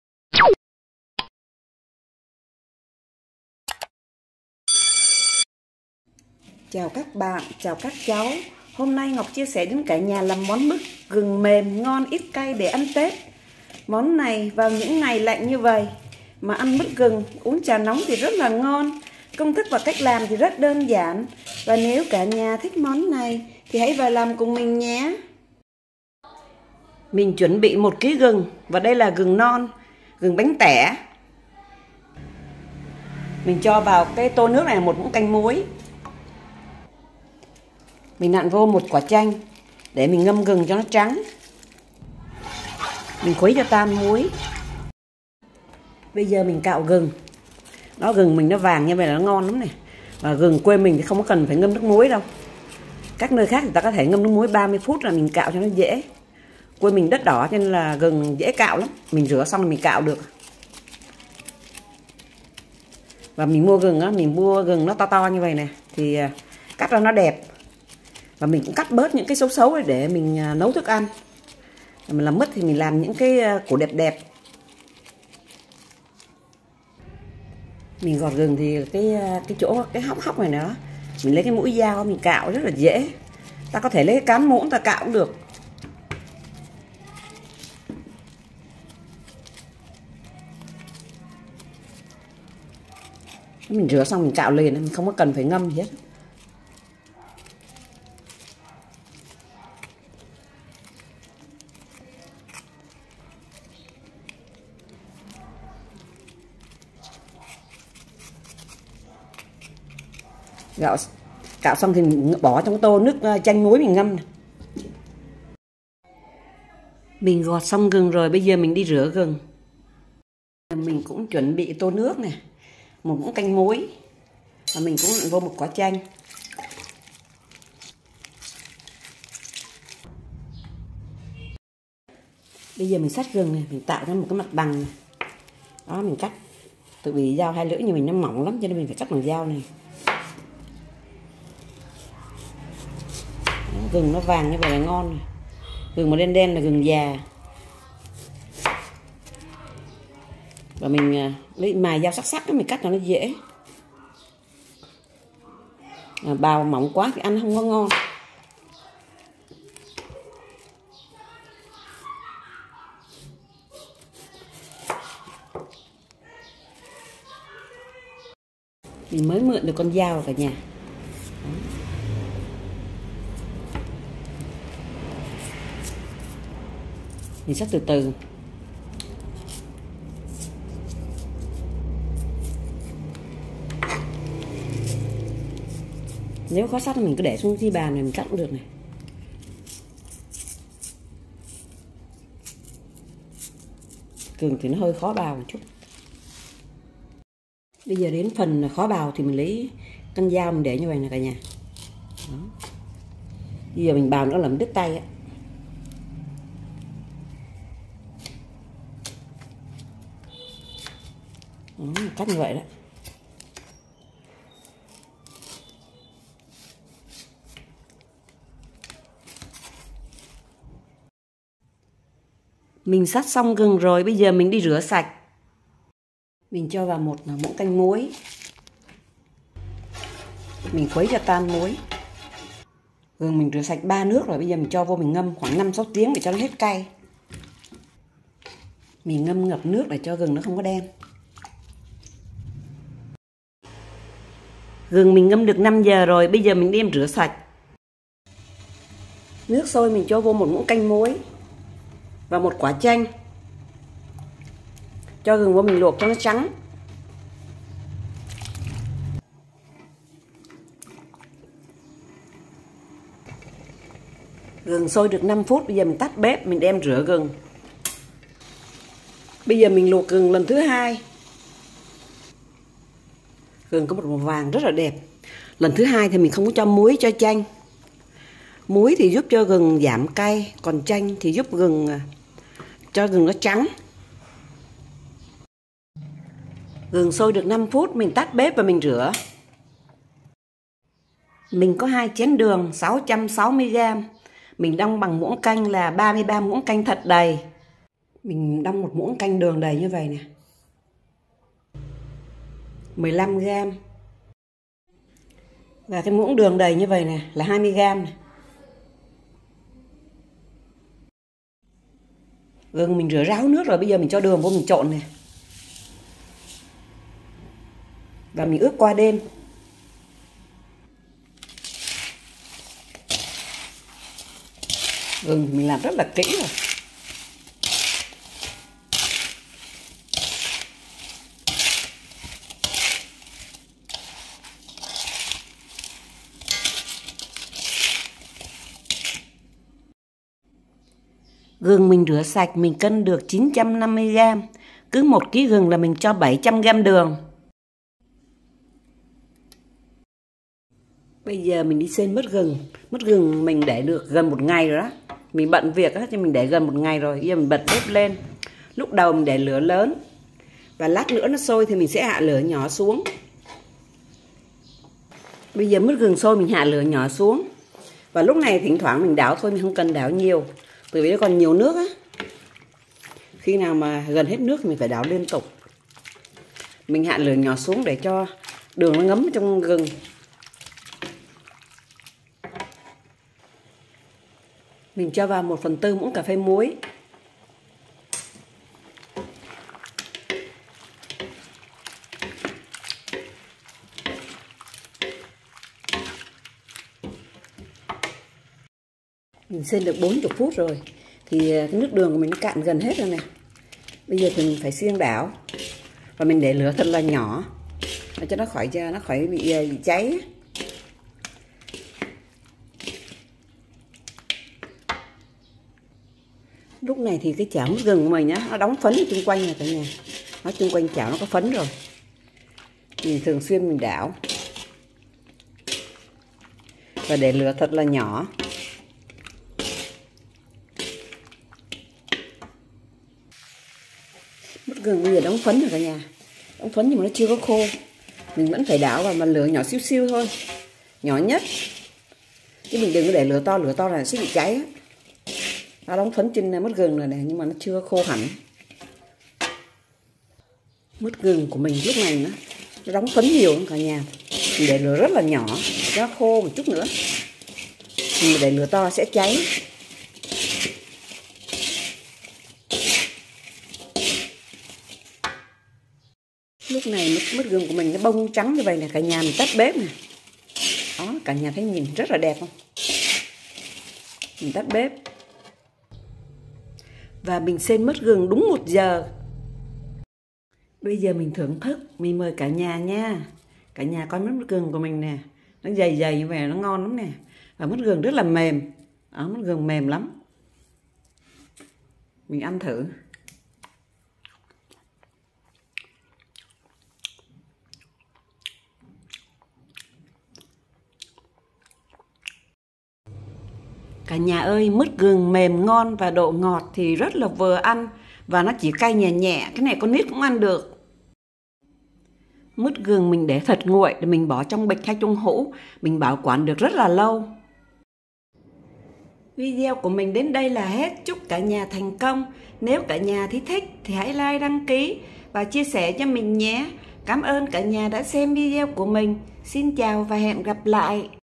LÀM MỨT GỪNG MỀM DẺO NGON ÍT CAY EM NHỎ CŨNG ĂN ĐƯỢC NGỌC NGUYỄN
CHIA SẺ CÔNG THỨC VÀ CÁCH LÀM MỨT GỪNG MỀM DẺO NGON ÍT CAY,CÔNG THỨC VÀ CÁCH LÀM RẤT ĐƠN GIẢN AI CŨNG LÀM ĐƯỢC
NGUYÊN LIỆU
1/ GỪNG 1KG
2/ ĐƯỜNG CÁT 660G
3/ MUỐI NGÂM GỪNG 1MC
4/ CHANH NGÂM GỪNG 1QUẢ
5/ MUỐI LUỘC GỪNG 1MC
6/ CHANH LUỘC GỪNG 1 QUẢ
7/ MUỐI SÊN MỨT 1/4 MCP GẠT